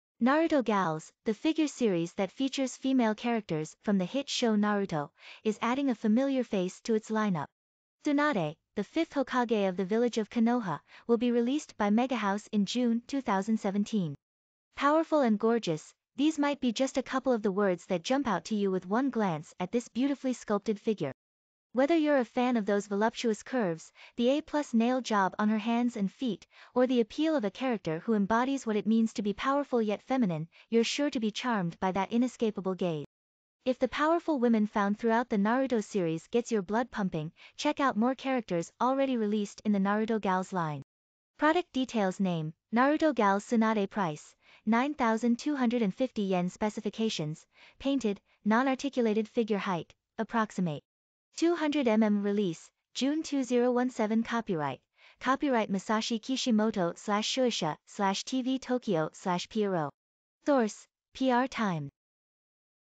Powerful Tsunade Is Next to Join MegaHouse's Naruto Gals Figure Series!
Naruto Gals, the figure series that features female characters from the hit show Naruto, is adding a familiar face to its lineup! Tsunade, the Fifth Hokage of the village of Konoha, will be released by MegaHouse in June 2017! Powerful and gorgeous - these might be just a couple of the words that jump out to you with one glance at this beautifully sculpted figure! Whether you’re a fan of those voluptuous curves, the A+ nail job on her hands and feet, or the appeal of a character who embodies what it means to be powerful yet feminine, you’re sure to be charmed by that inescapable gaze! If the powerful women found throughout the Naruto series gets your blood pumping, check out more characters already released in the Naruto Gals line. Product Details■ Name: Naruto Gals Tsunade■ Price: ¥9,250 ■ Specifications: Painted, non-articulated figure■ Height: approx. 200 mm■ Release: June 2017■ Copyright: ©Masashi Kishimoto / Shueisha / TV Tokyo / Pierrot Source: PR TIMES